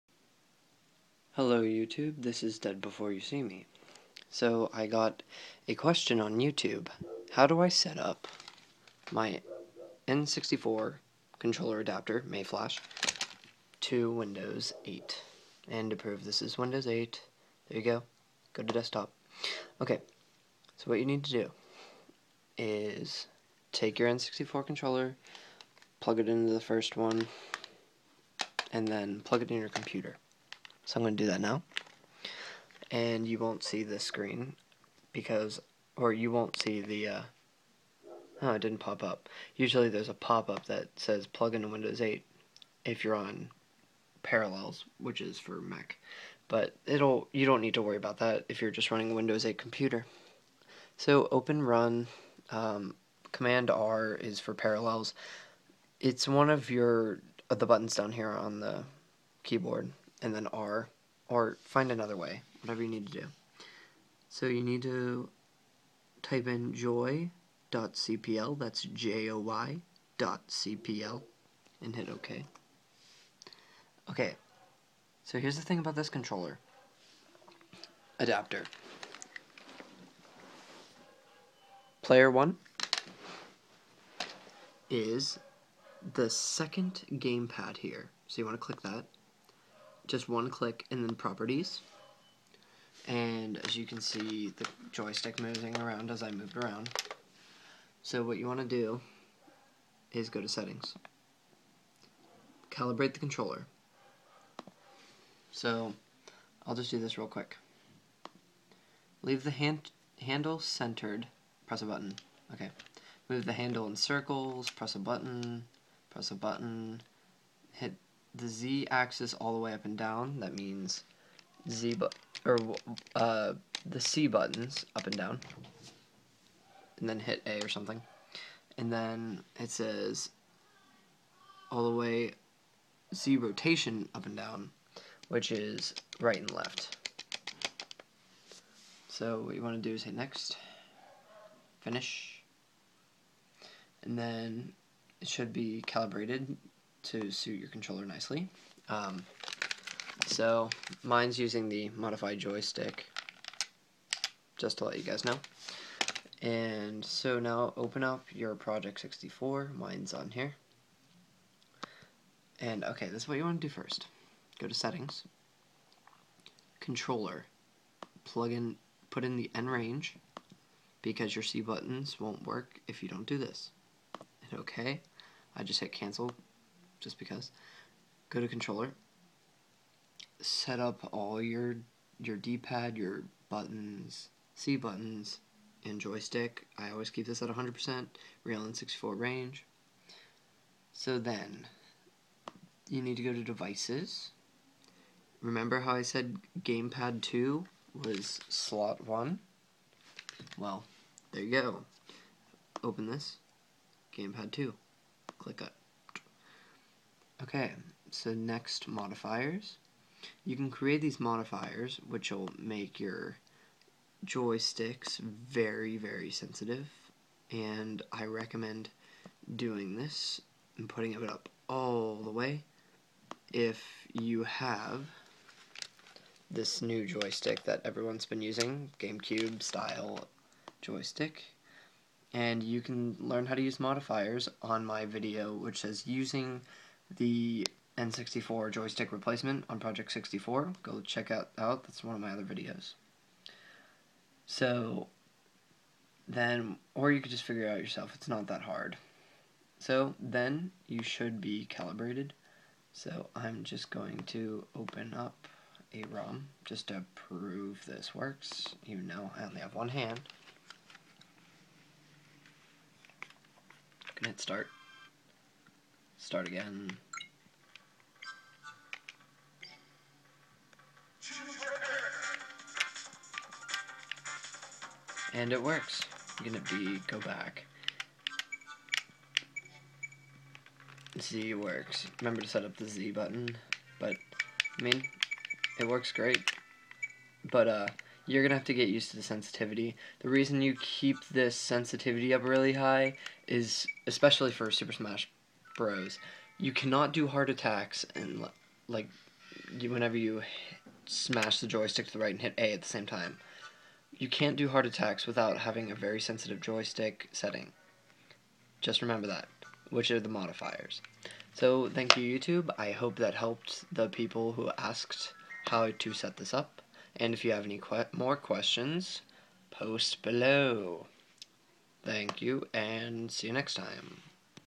How to set up the mayflash n64 controller adapter on windows 8
Mayflash on project 64 for windows 8. my old video showed windows do if you need that one.